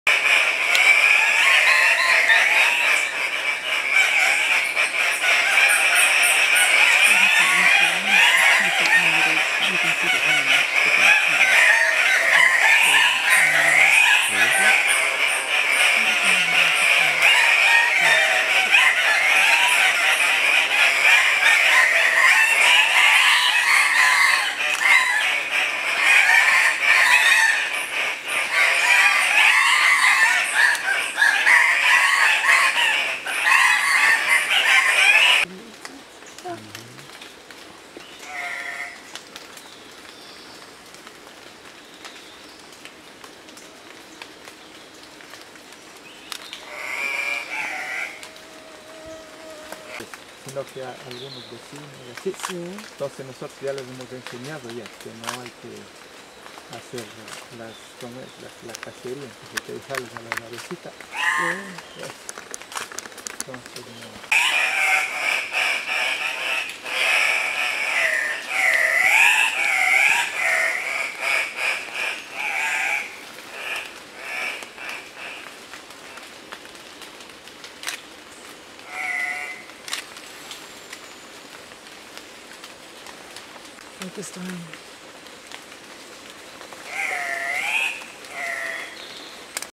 Andean Cock-of-the-Rocks at morning lek
These male Andean Cock-of-the-Rocks were in full display mode at a lek near Pacto Sumaco, Napo, Ecuador on 15 February 2015